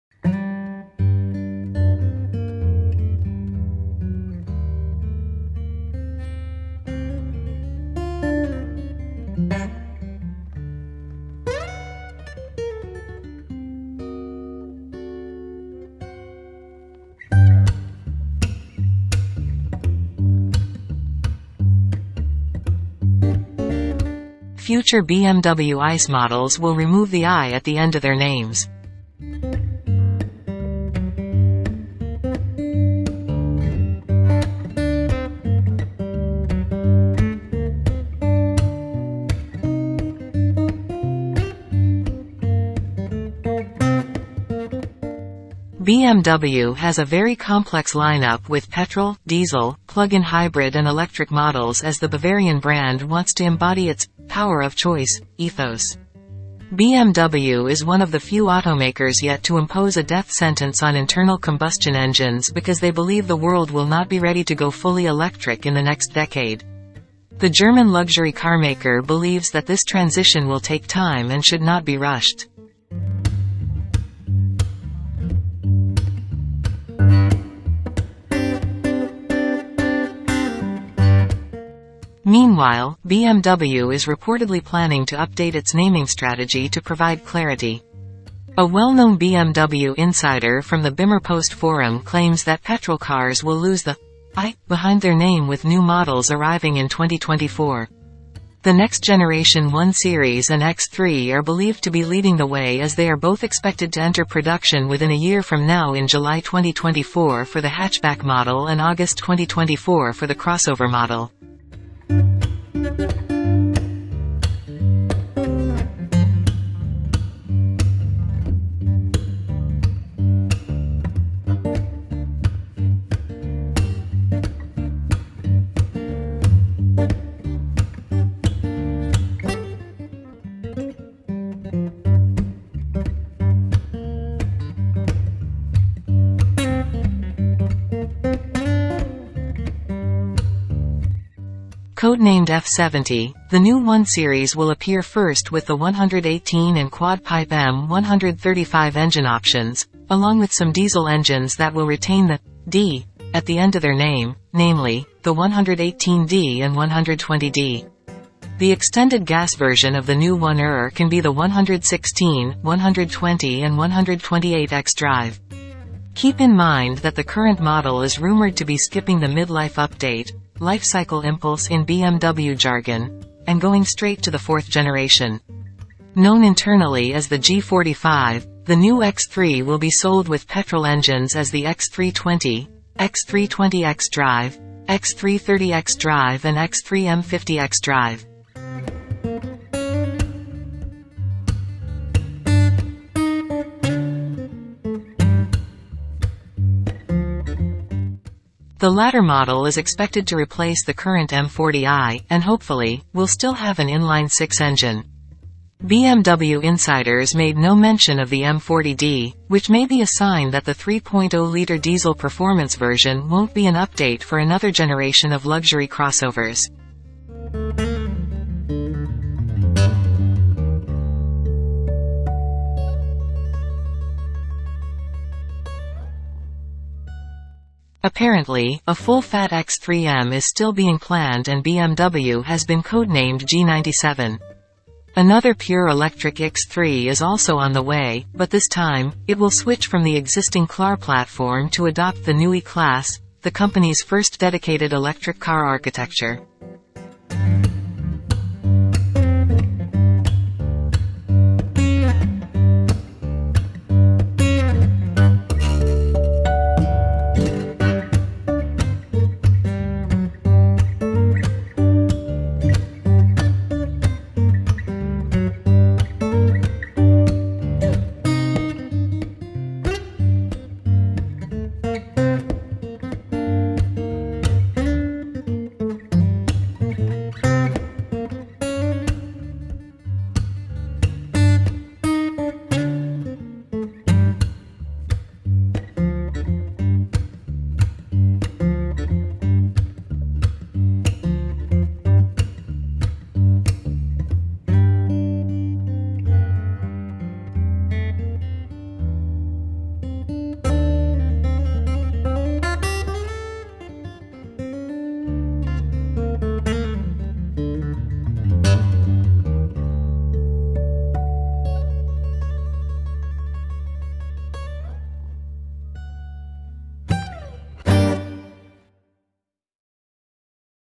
Future BMW ICE Models Will Remove the i
BMW has a very complex lineup of petrol, diesel, plug-in hybrid and electric models as the Bavarian brand looks to refine its range.

 BMW is one of the few automakers yet to issue a death sentence on internal combustion engines because they say the world is not ready to go fully electric in the next decade.